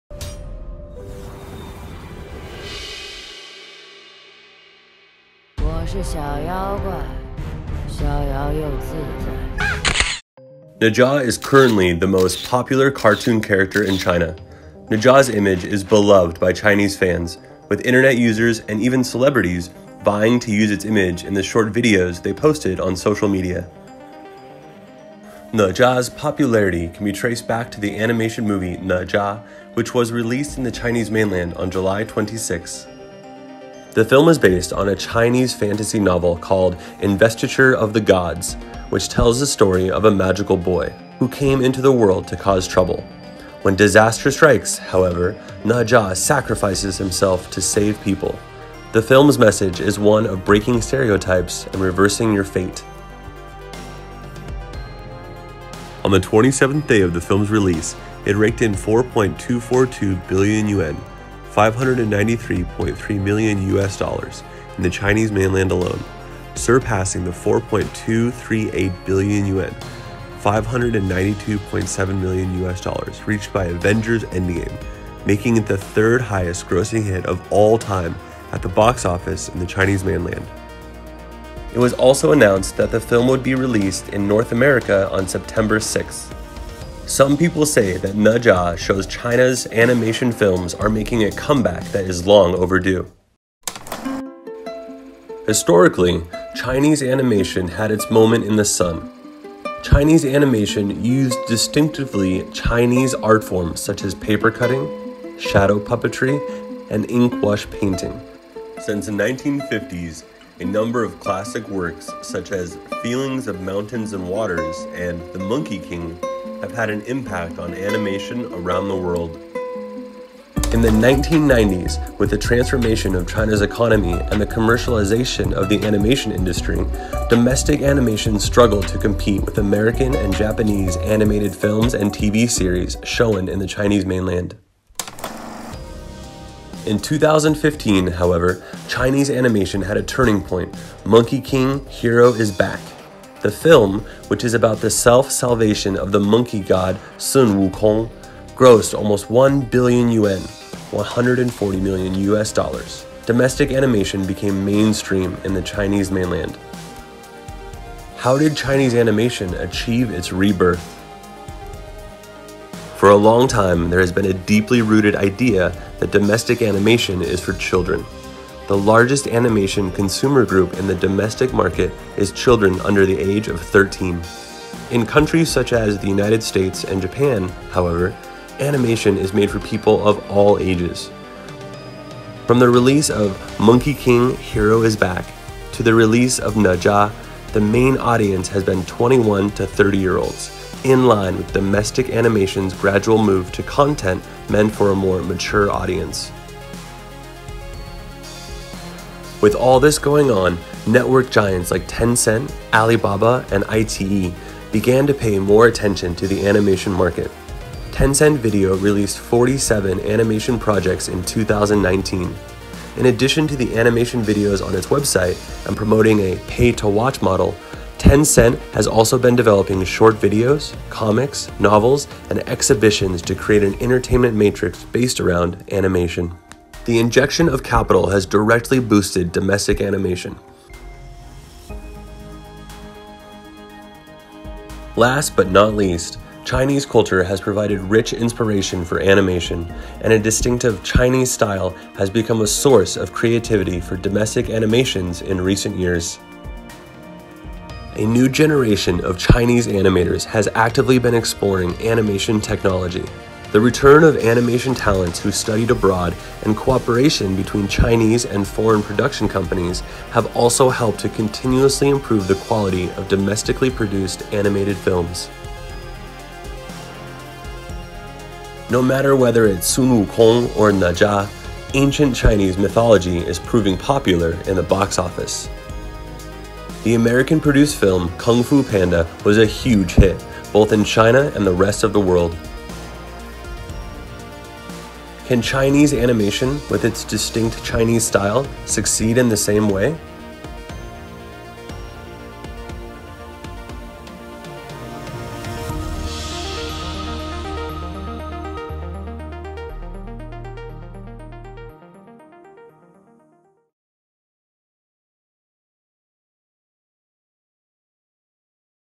Nezha, the hottest animation film in China
What does the most successful Chinese animation film of all time mean for the country's animation industry?